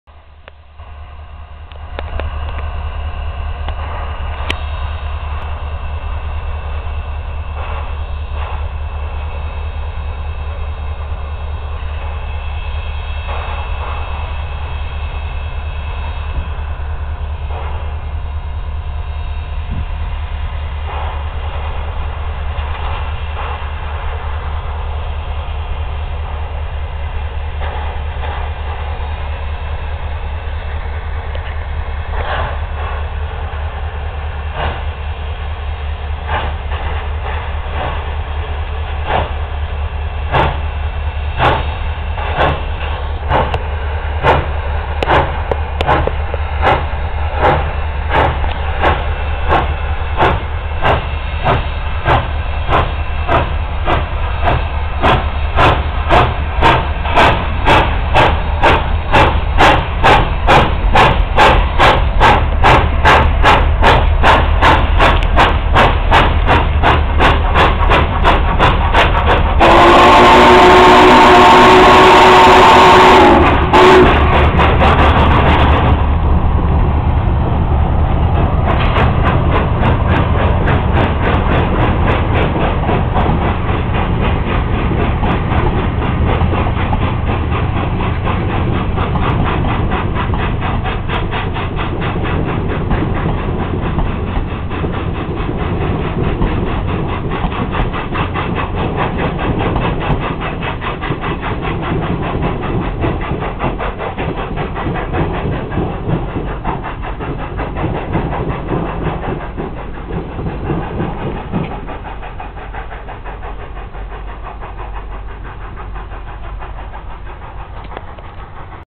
Union Pacific #844 in Falls City, NE
Stopped in Falls City, NE on June 11'th, 2011.